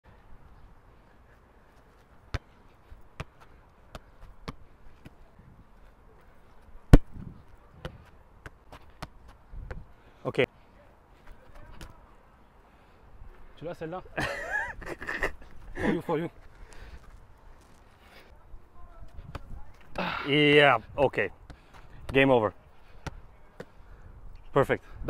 Rating Camavinga's touch
We met up with Eduardo Camavinga and of course had to test his touch - let us know if you agree with our ratings!